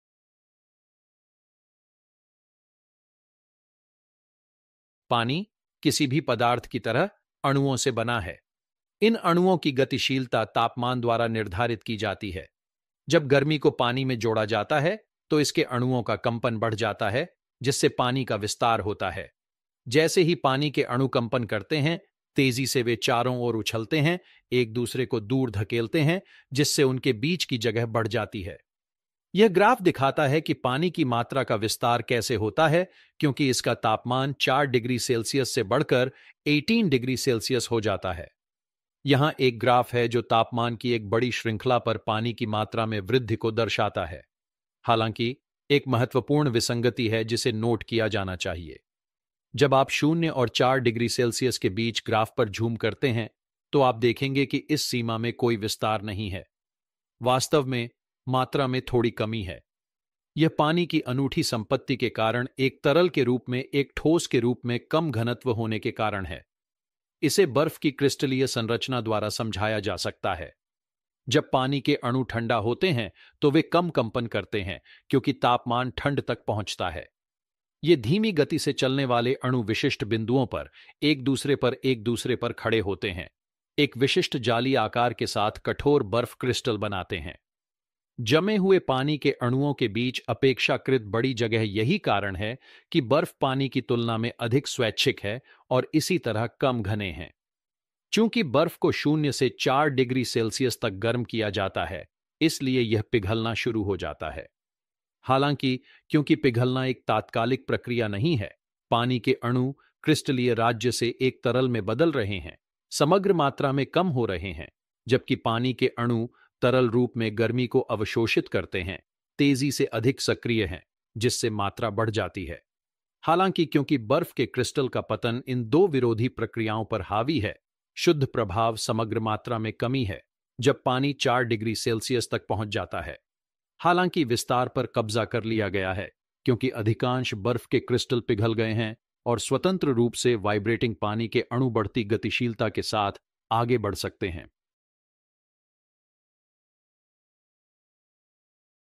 Temperature 🔥 and Water💦
Hello Everyone
in this video we are going to learn the relationship between Heat and Water. What Happened when water is being heated. We will see the step by step behavior of water when the temperature rises. We have produced some High quality Animations and used 3d technology for better demonstration to you.